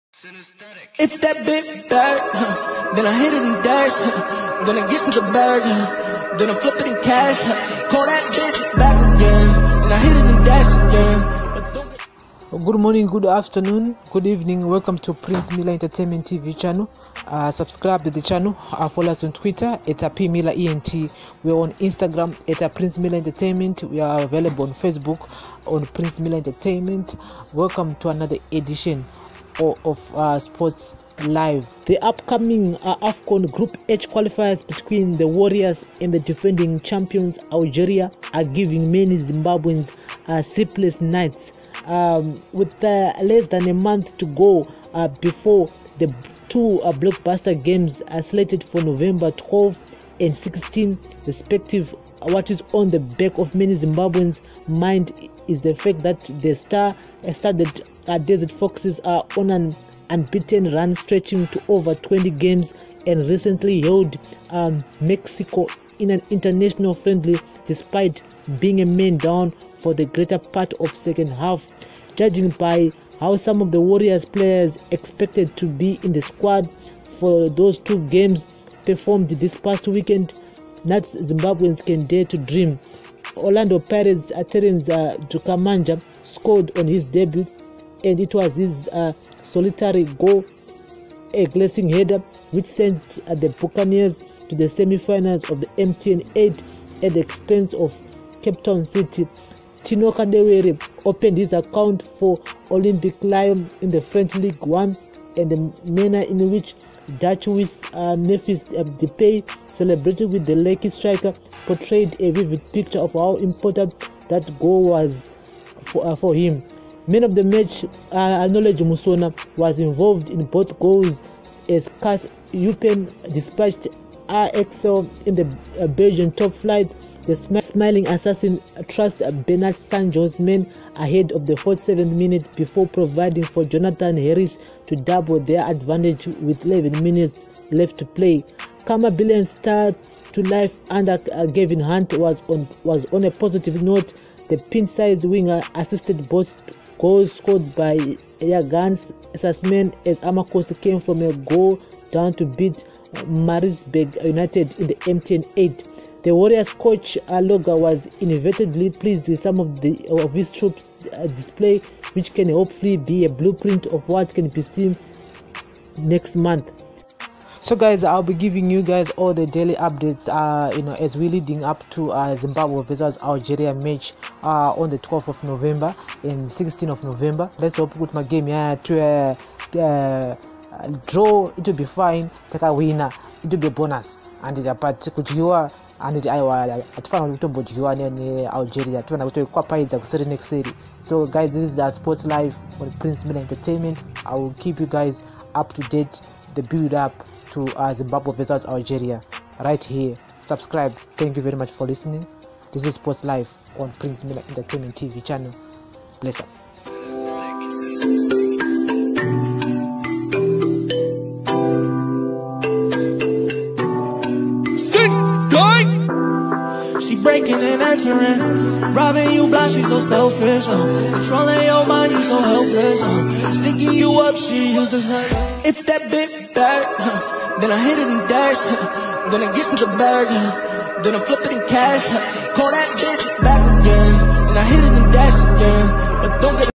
• WARRIORS SHARPENS AHEAD OF ALGERIA BATTLE | SPORTS LIVE EP11
Sports Live Episode Update on Warriors Stars having a good weekend in office ahead of the crucial feature with Algeria in November
Lets Talk In the comment section